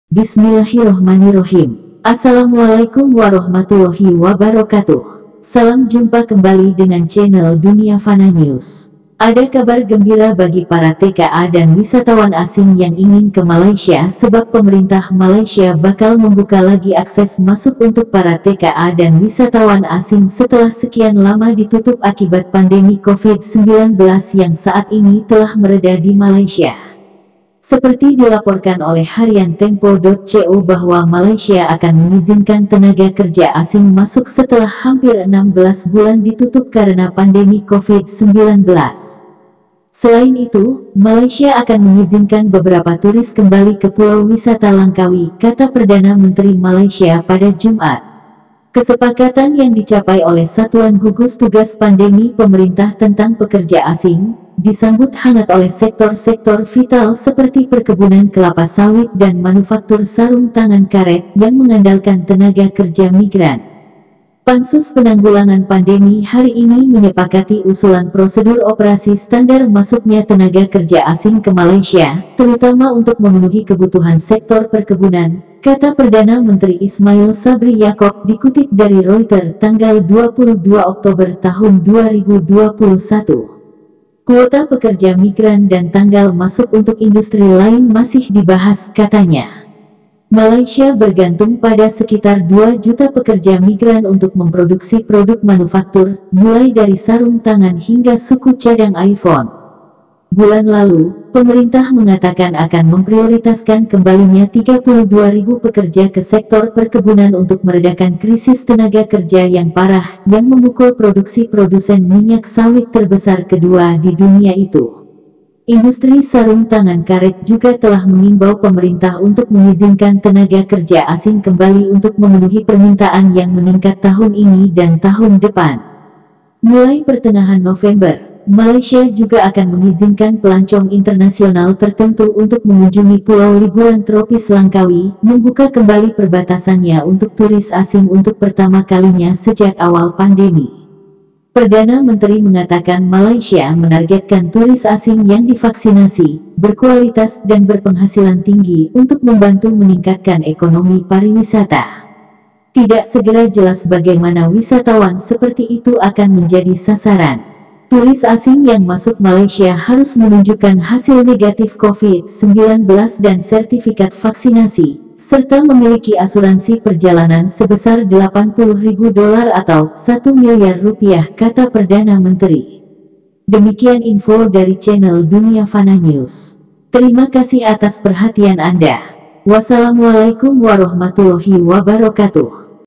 THANK YOU MALAYSIA BAKAL IZINKAN TKA DAN TURIS ASING MASUK LAGI 🙏 (melayu subs)
sumber info tempo.co,foto pinterest